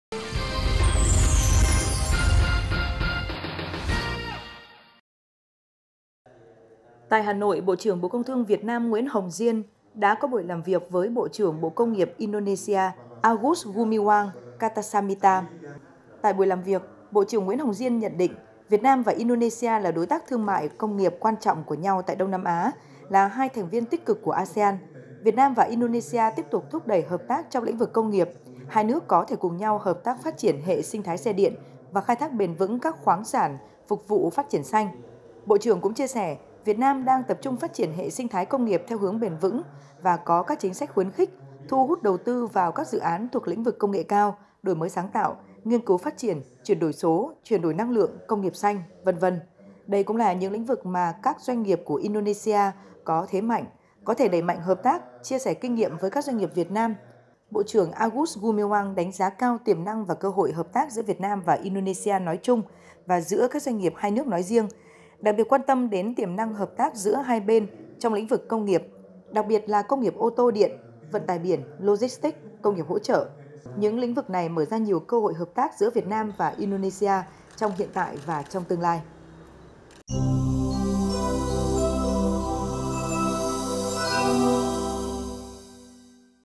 Việt Nam - Indonesia: Thúc đẩy hợp tác thương mại, công nghiệp
Tại Hà Nội, Bộ trưởng Bộ Công Thương Việt Nam - Nguyễn Hồng Diên đã có buổi làm việc với Bộ trưởng Bộ Công nghiệp Indonesia – Agus Gumiwang Kartasasmita…